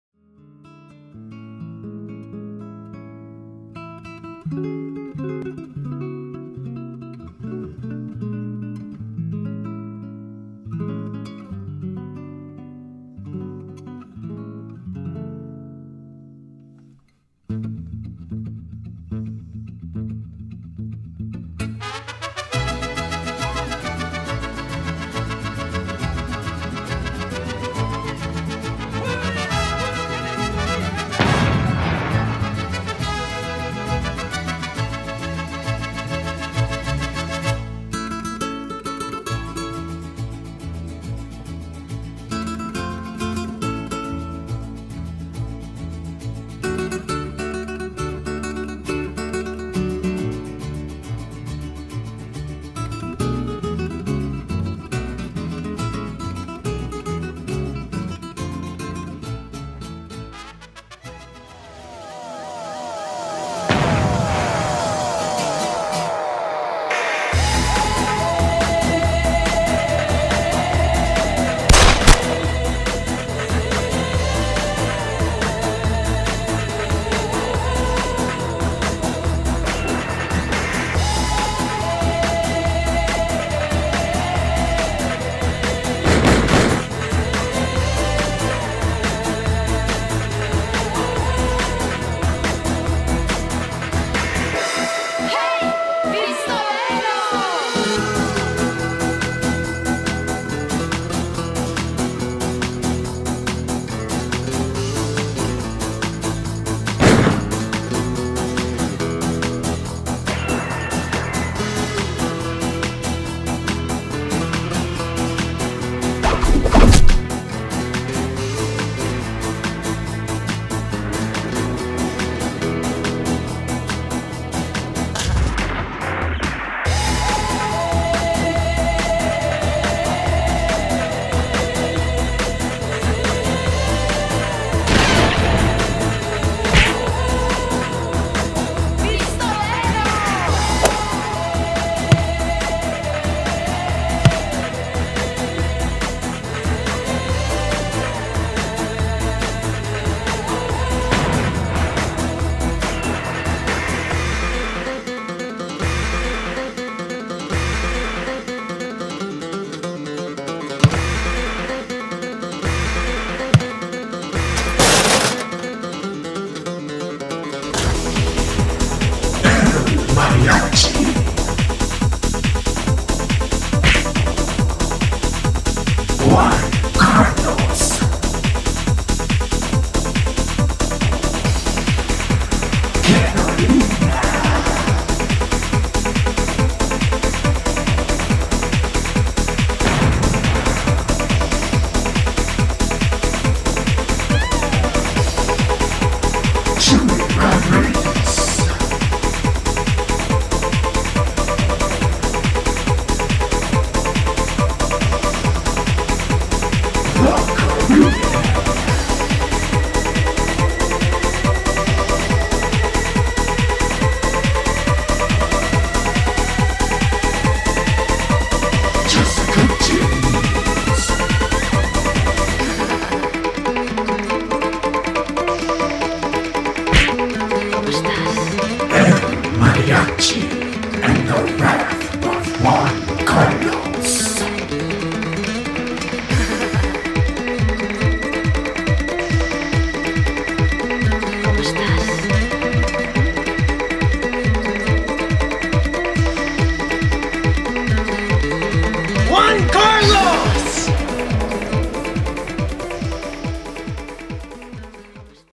El Mariachi Trailer
This is a preview for my upcoming movie, "El Mariachi and the Wrath of Juan-Carlos". It's an action-packed, epic story of a man on a mission to rescue his love from the clutches of evil.